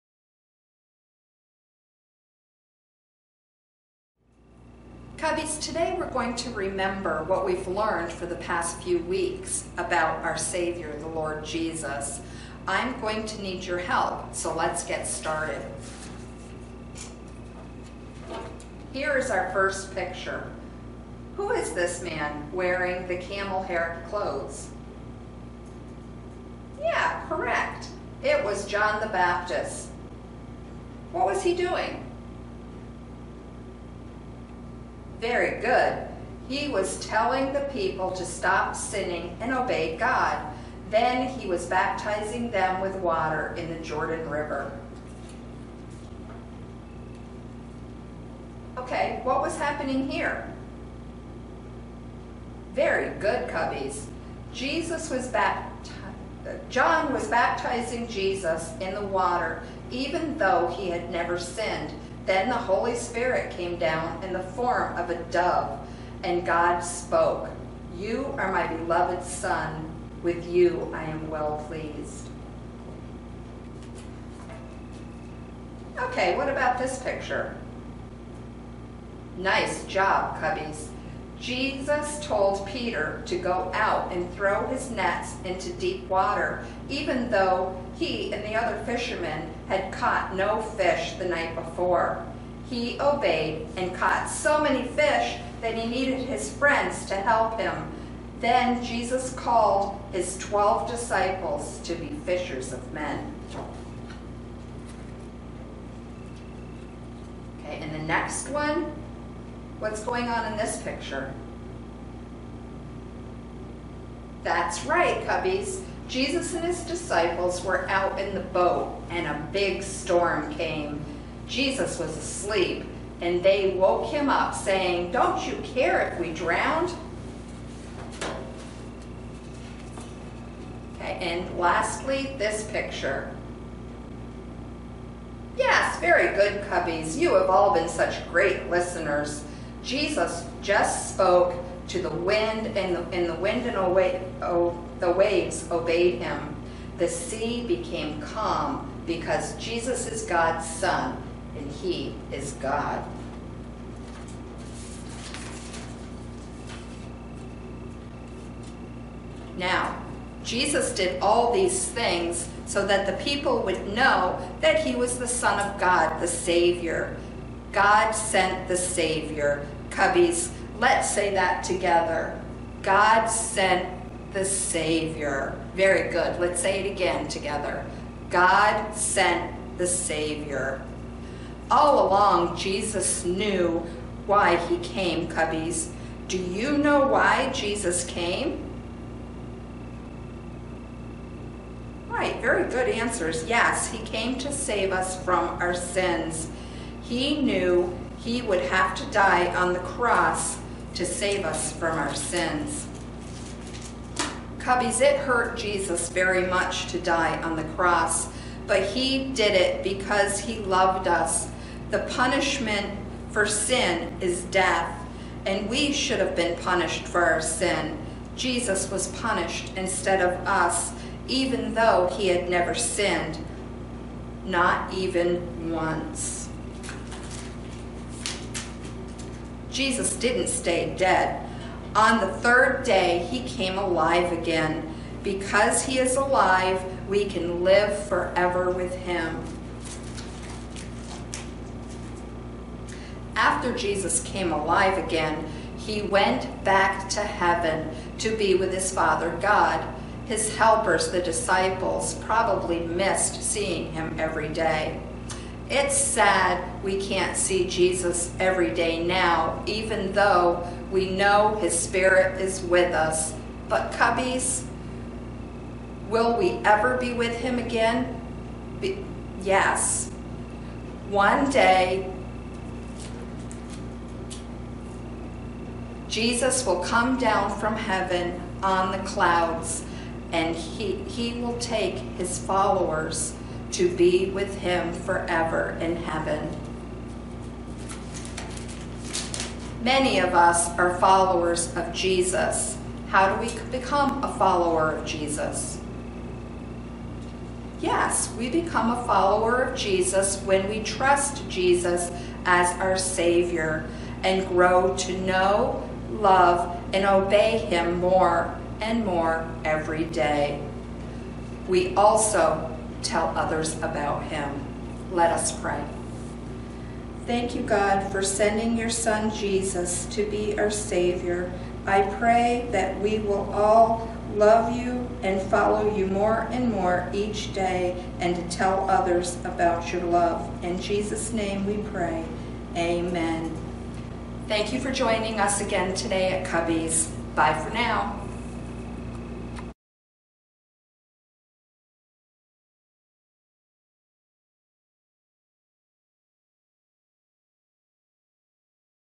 Appleseed BH26 Lesson
The Bible lesson from the Awana Cubbies Appleseed book, Unit 6, Bear Hug 26 - Produced by Commander Bill @ Hanover Fellowship Church - commanderbill.net